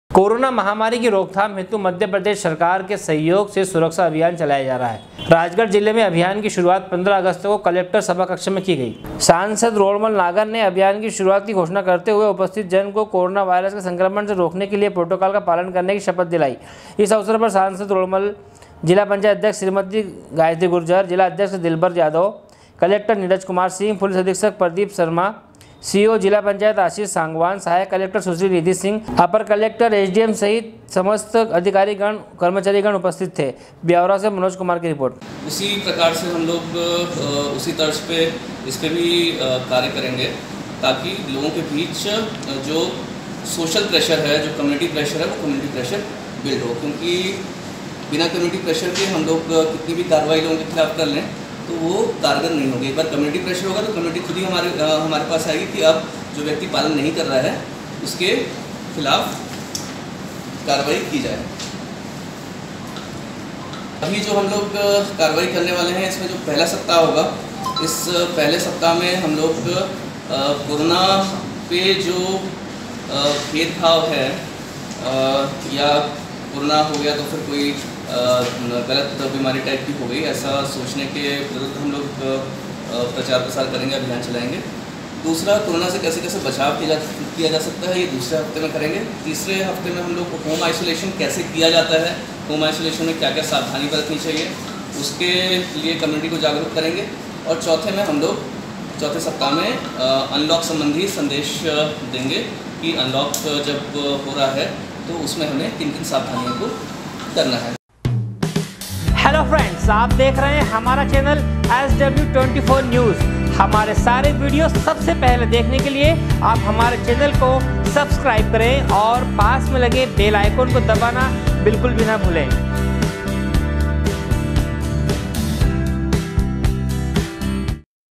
SW 24 News Madhyapradesh
खरगोन जिले में स्वतंत्रता दिवस के अवसर पर जमाअत इस्लाहुल मुस्लेमीन के सदर हाजी सिराजुद्दीन शेख ने जमाअत के कार्यालय पर झंडावंदन किया गया। खरगोन से अजय तिवारी की रिर्पेाट